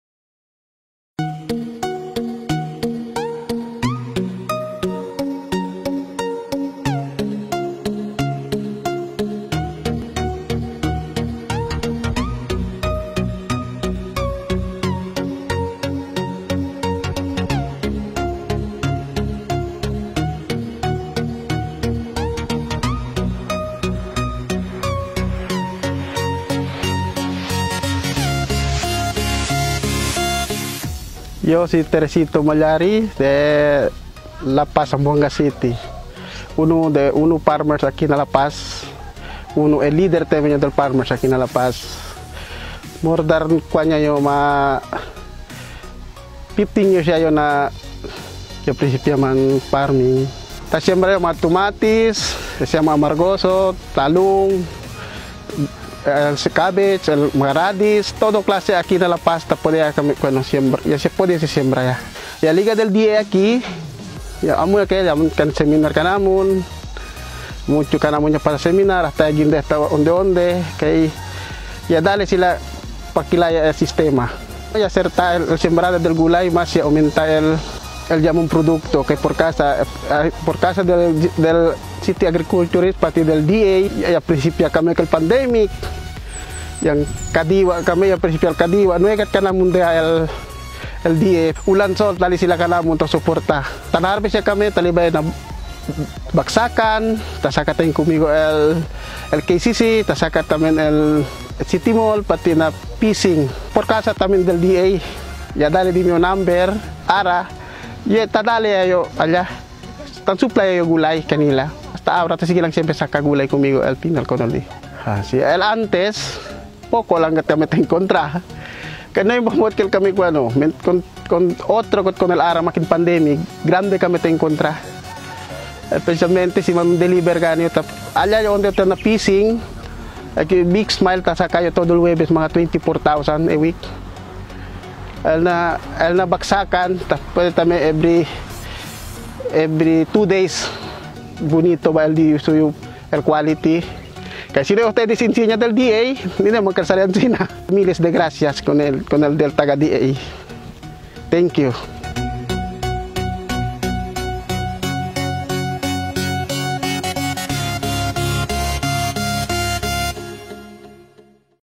One Happy Farmer - A HVCDP Support Project and KADIWA ni Ani at Kita
A video testimony of Teresito "Toy" Mallari of barangay La Paz in Zamboanga City who shares how the Department of Agriculture helped him in his vegetable production through the High Value Crops Development Program (HVCDP) Seeds assistance and the DA-AMAD Market linkages through the KADIWA ni Ani at Kita.